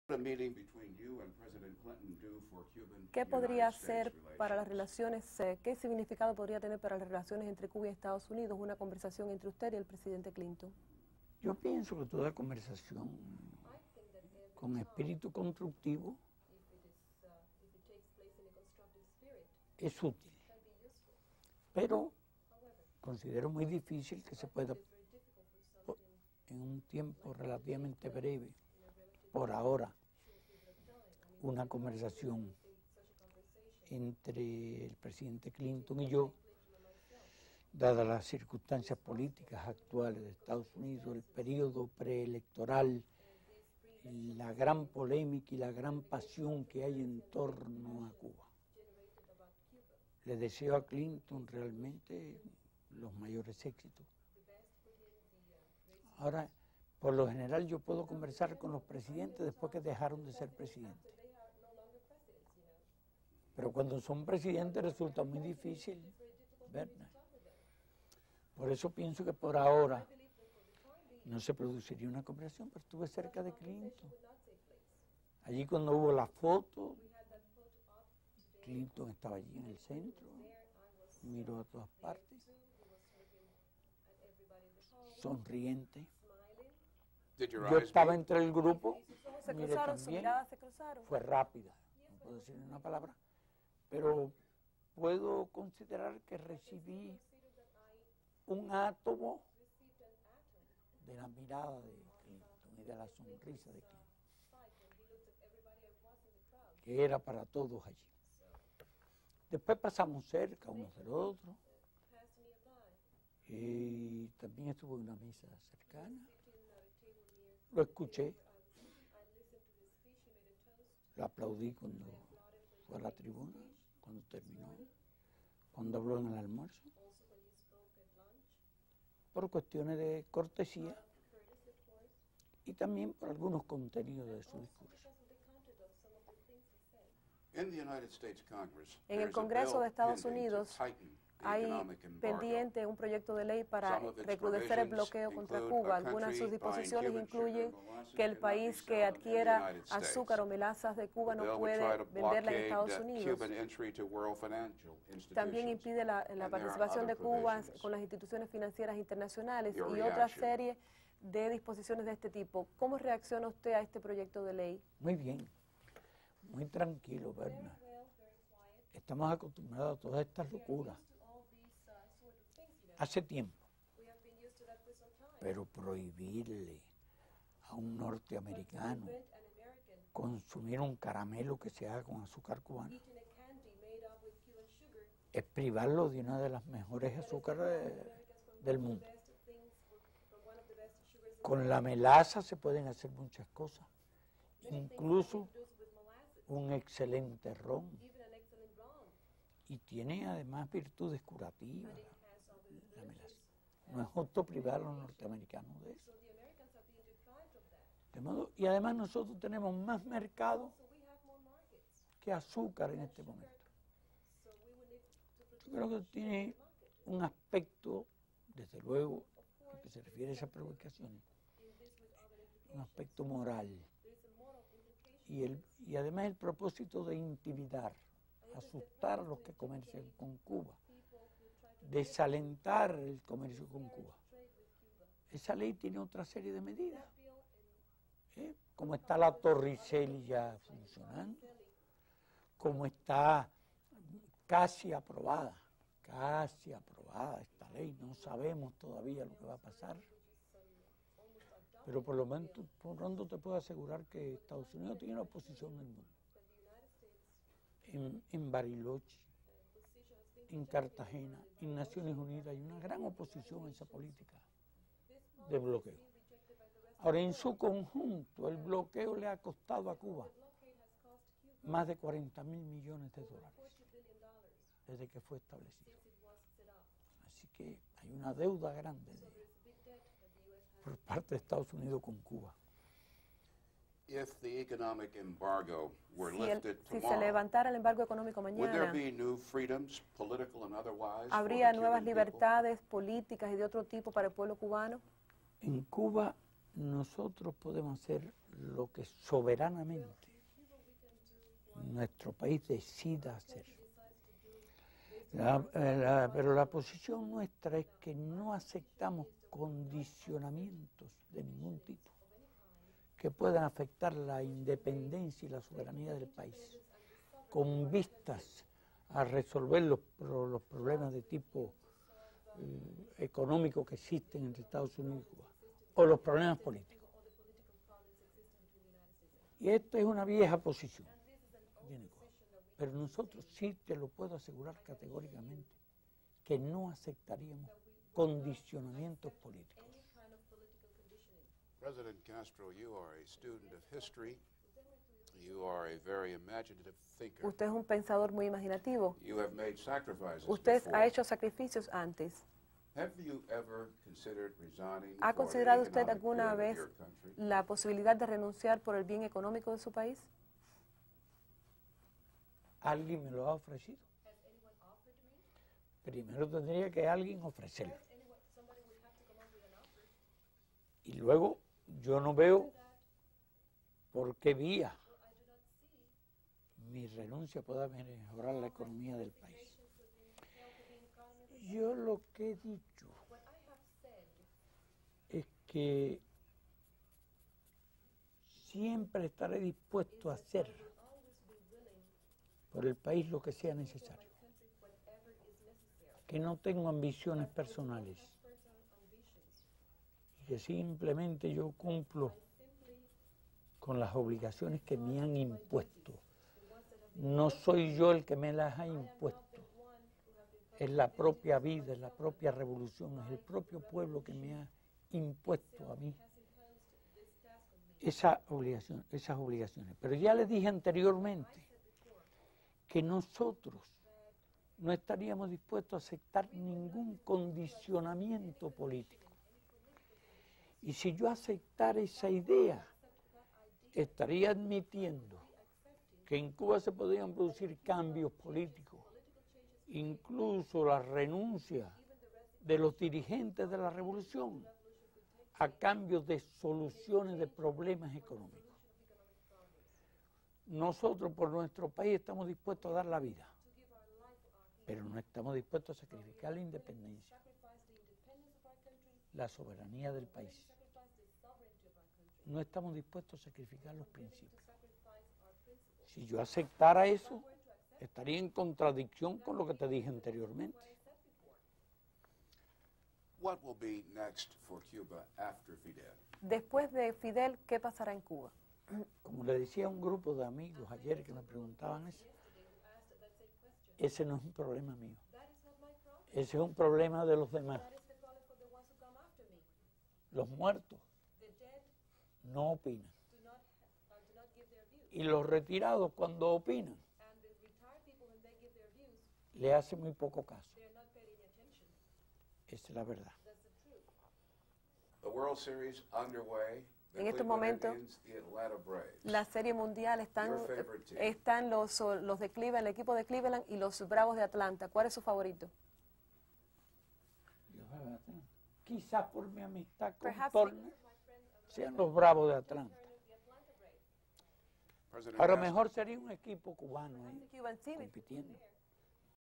Entrevista de Bernard Shaw a Fidel Castro
Fragmento de la entrevista concedida a Bernard Shaw, periodista de la cadena de televisión norteamericana CNN.